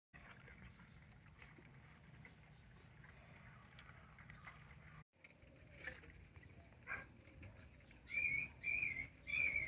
Hairless sempre menores. Mas desenvolvendo bem. 17 dias dos filhotes de rato camundongo.
Os filhotes de rato camundongo estão com 17 dias.
Muito fofinhos!!!

Os Hairless são sempre menores, mas estão desenvolvendo bem.
Eles já conseguiram abrir os olhinhos iguais aos outros.

O difícil é gravar vídeo agora 😂.
Não param mais quietos ❤️.

Mas vamos continuar.

gratidão ☺️

Deus abençoe 🙌🏼